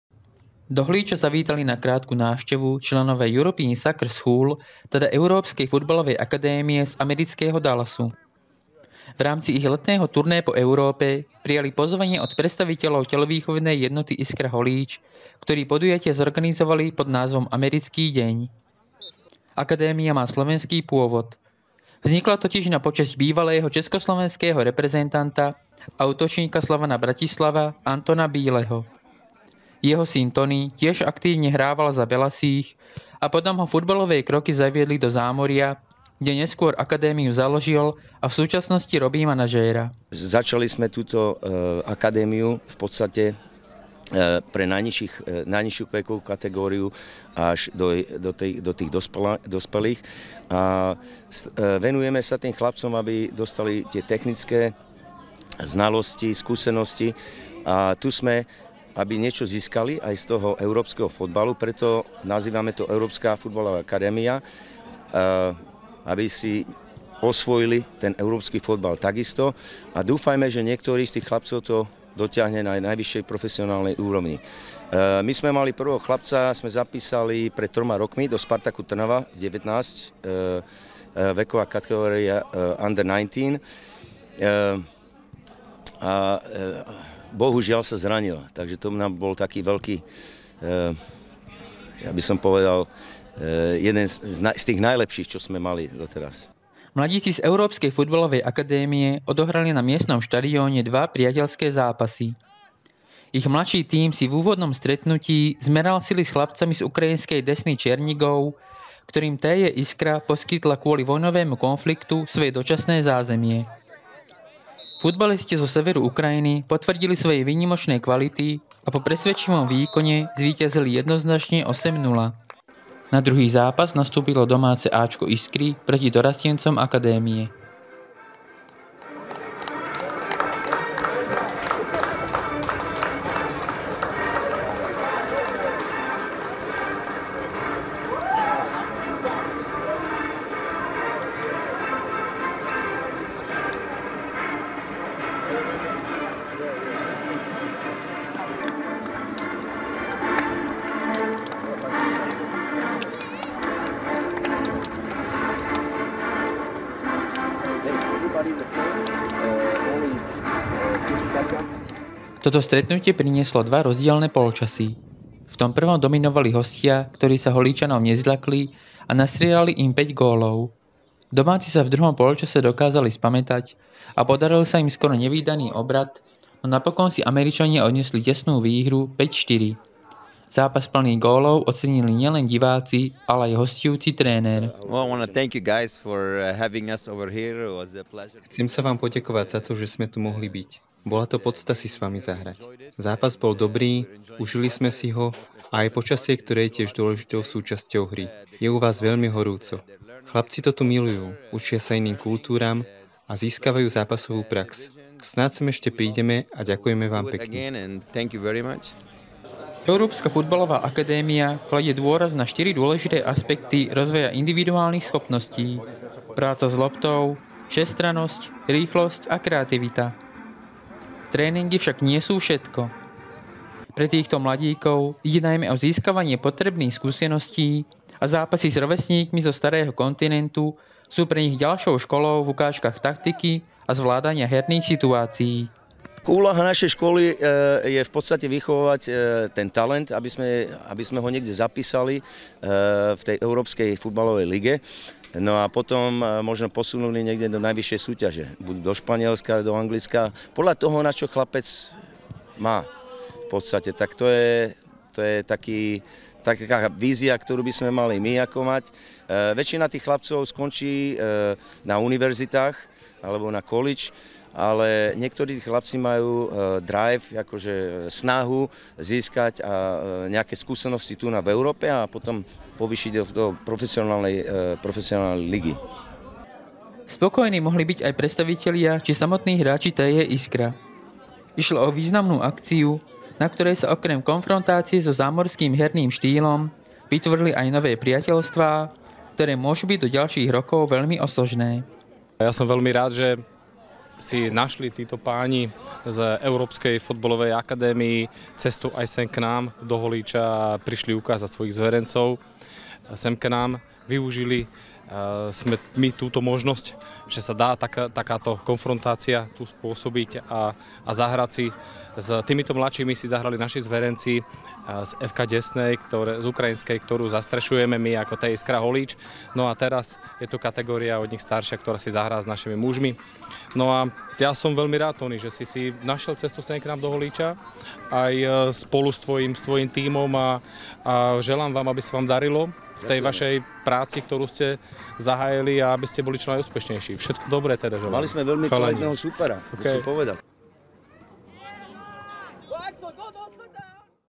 Futbalová návšteva zo zámoria
V rámci letného turné si prišla do Holíča zahrať Európska futbalová akadémia z amerického Dallasu. Na štadióne TJ Iskra odohrali mladíci dva priateľské zápasy. Akadémia má slovenský pôvod a snaží sa z chlapcov vychovať budúcich profesionálnych futbalistov v špičkových európskych ligách.